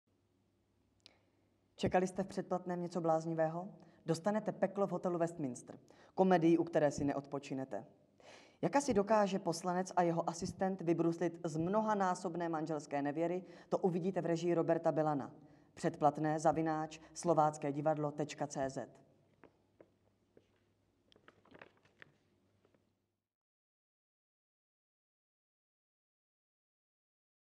David Macháček - Peklo v hotelu Westminster
Herec David Macháček o komedii Peklo v hotelu Westminster.